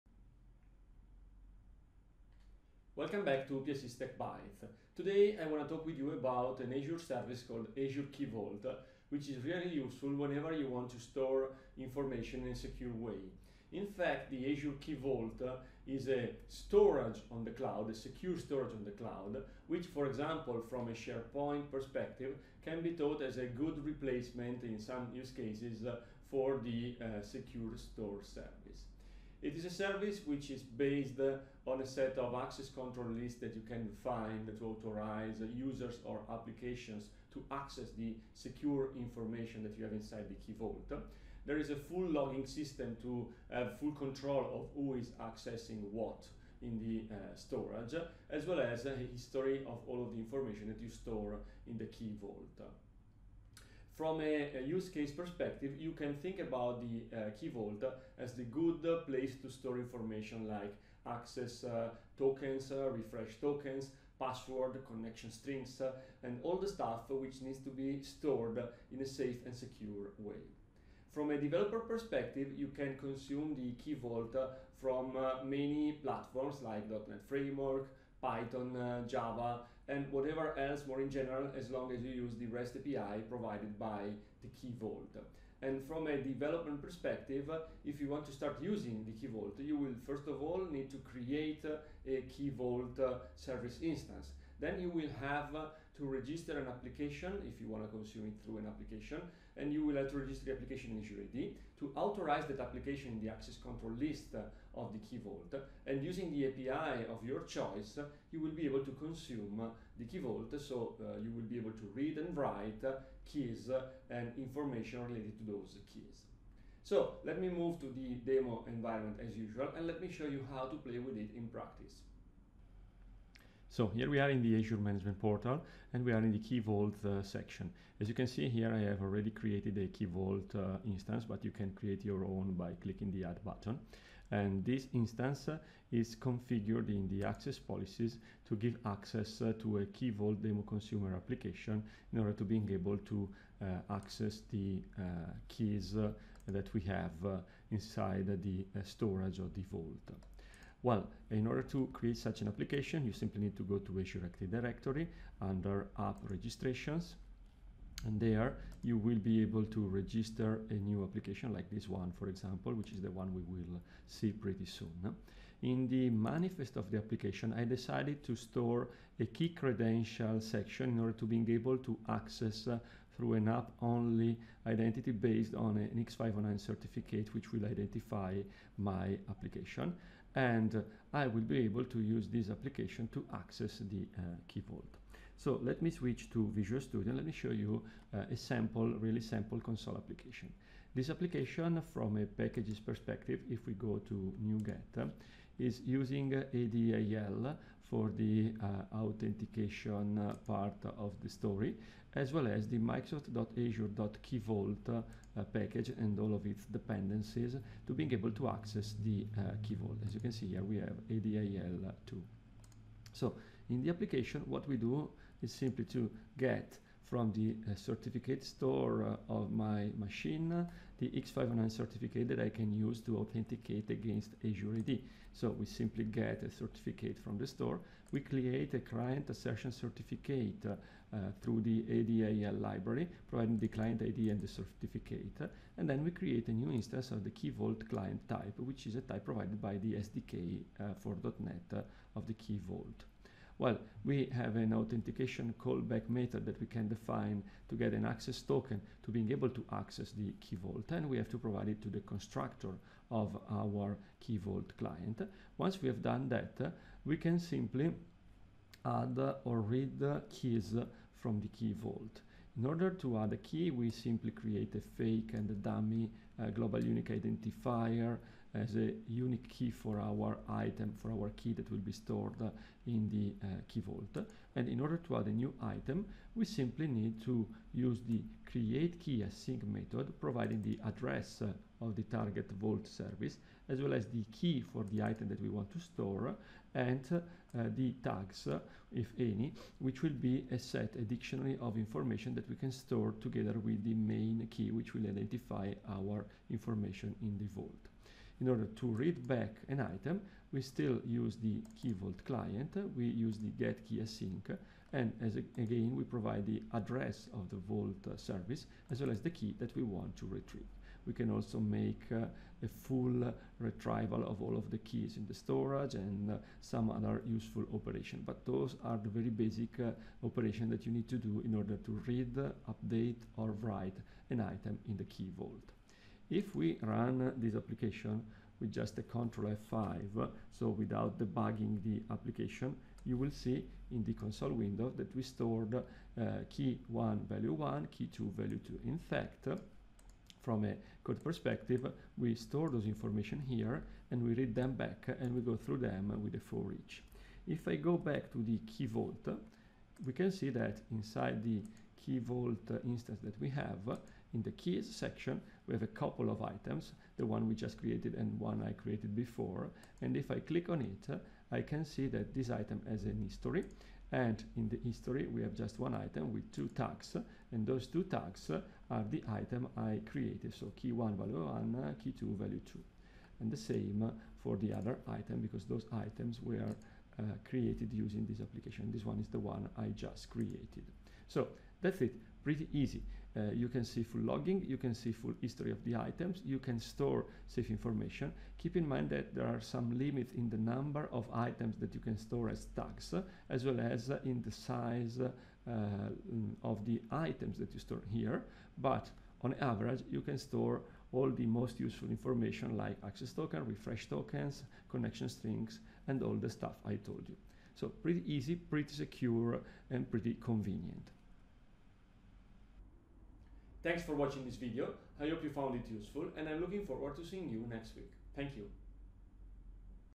Episode #27 - Storing Keys and Secrets in the Azure Key Vault
Learn how to leverage the Azure Key Vault service to store keys and secrets (Access Token, Refresh Tokens, Passwords, etc.) in a secure and monitored storage on the cloud.